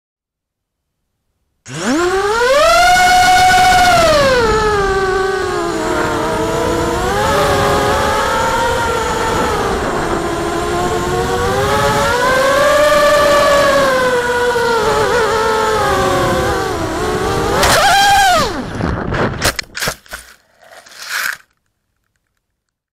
Not everything works like a charm...QAV250 - 4 cell - 2300kv Cobra - CC3D Atom
20 sec of FUN with my screaming miniquad.

##Details&Parts List:###
QAV250 Carbon

Flown in "Acro+" on roll and pitch, yaw was "rate".
Flighttime: approx. 5min with this style of flying.